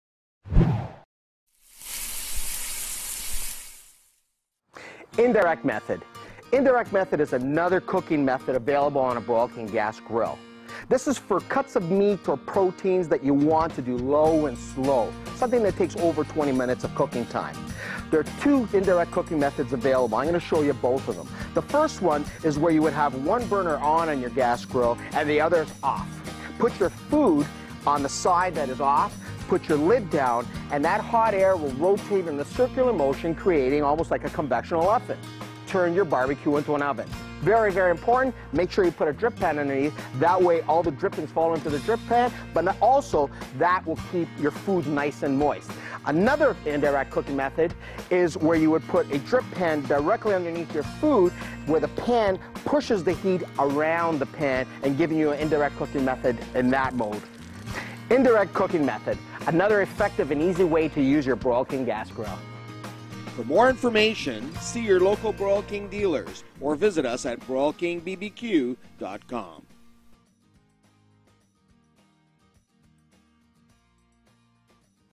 Broil King Techniques: Indirect Grilling
In this segment BBQ Naz shows you how to grill using the Indirect method. Indirect cooking is great for cuts of meat that take a while to cook. The indirect, convection oven like way the heat circulates around the oven keep the food moist. Watch and enjoy!